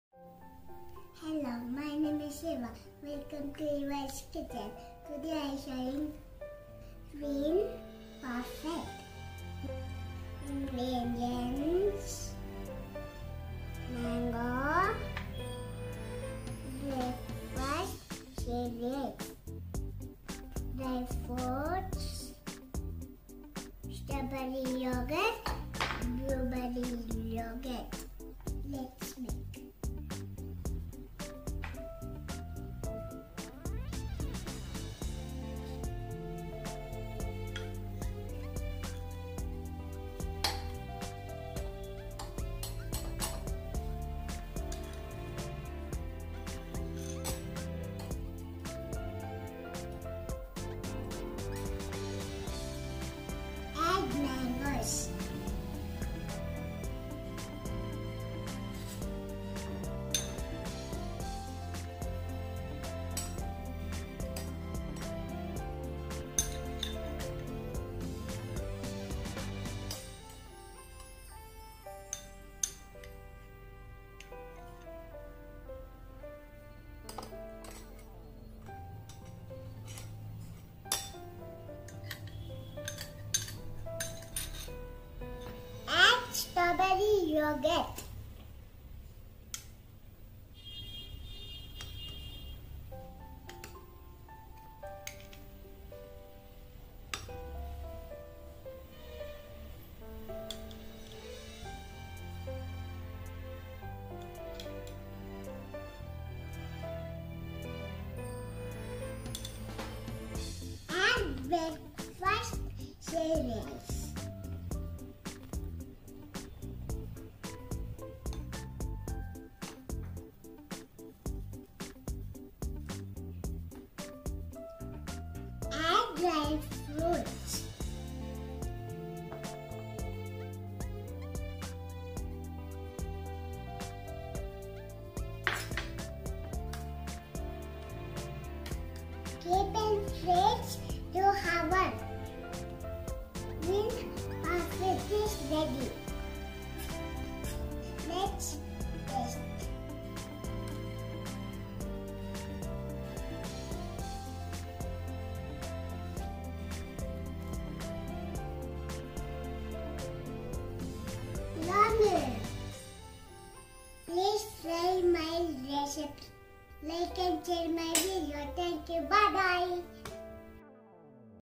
Twin Parfait | Healthy Receipe | 3yrs old Toddler Chef Eva
Ingredients

Mango - 1/2 c
Strawberry yogurt - 3 tbsp
Blueberry yogurt - 3 tbsp
Breakfast cereals - 2 tbsp
Dry fruits - 2 tsp